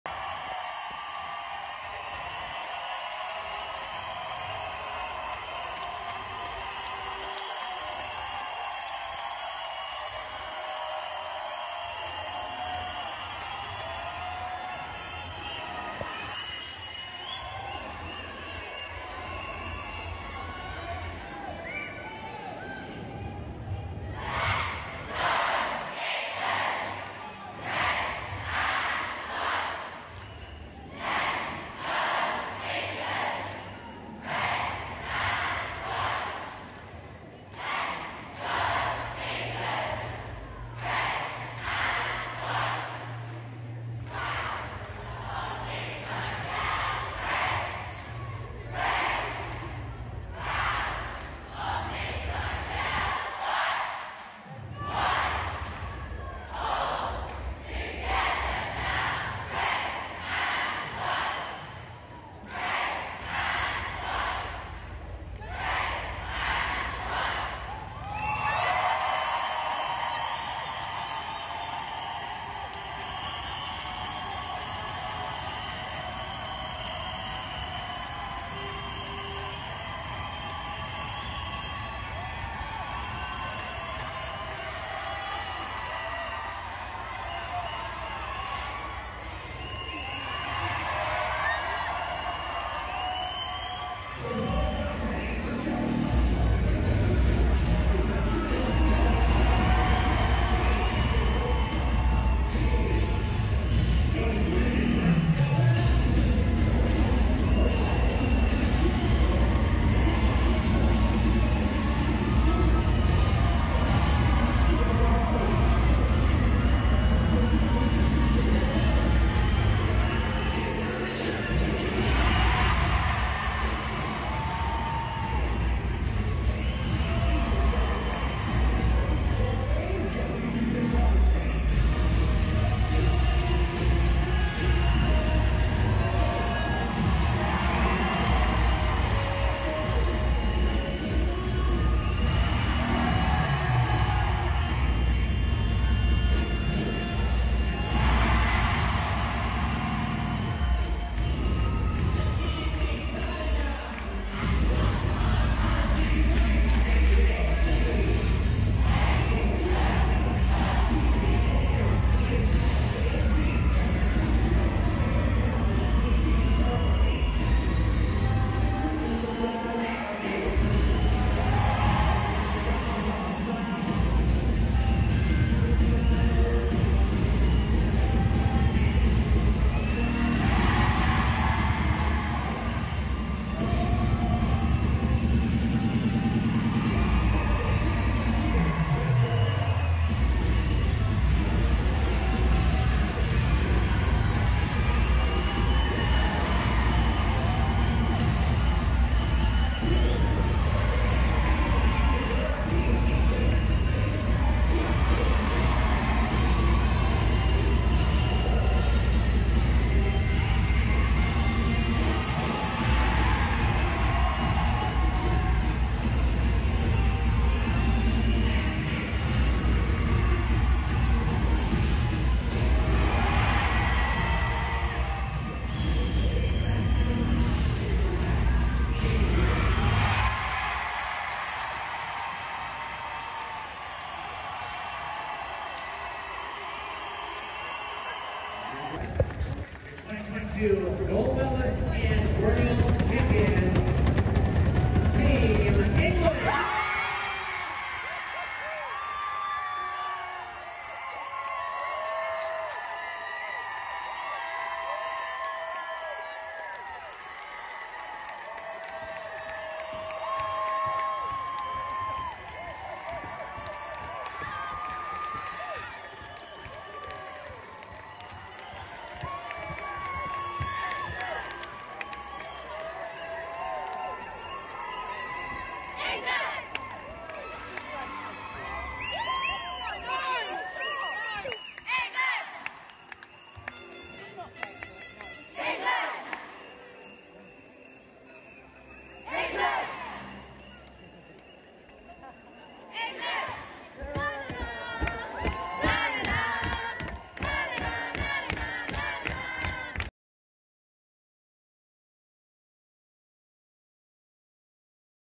UK cheerleaders win gold for England - making them the best in the WORLD | SWNS
A group of cheerleaders took top prize competing for England in an international competition - making them the best in the WORLD.

England's Youth All Girl Median team of cheerleaders competed in the 2022 World Cheerleading Championship in Florida on April 20.

They completed an incredible routine for judges of the competition, which saw people somersaulting, flipping and flying through the air.

The all-girl team beat teams from all around the world in the competition - which is seen as the 'world cup of cheerleading' - and secured the title of world champions for England.